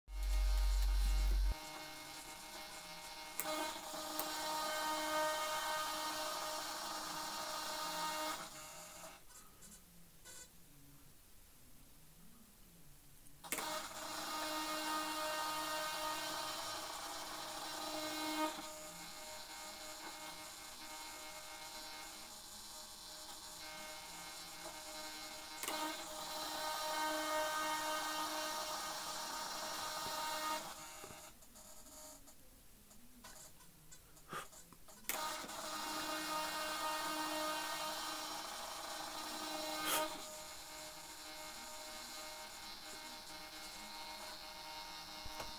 Folwer Flaps on the P-38 Lightning from VQ Model
The P-38 from VQ Model features functioning fowler flaps. These flaps move backwards and then down.